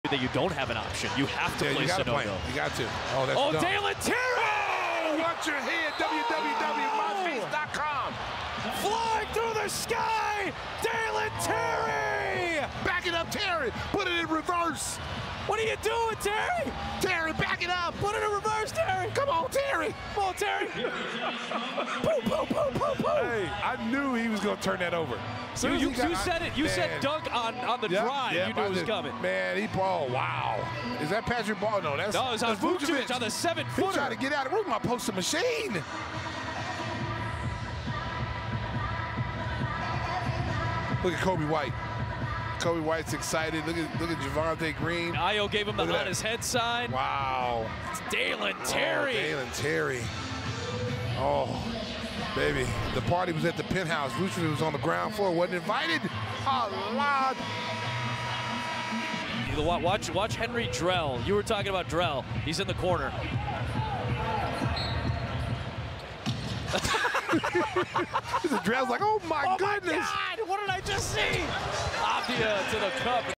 Dalen Terry with a massive poster dunk on Tristan Vukcevic! Drell cannot believe it! 4/12/24
What a poster dunk in the clutch!
Henri Drell is with the OMG like Mozgov back then, reacting to Larry Nance Jr. dunk on Brook Lopez back then.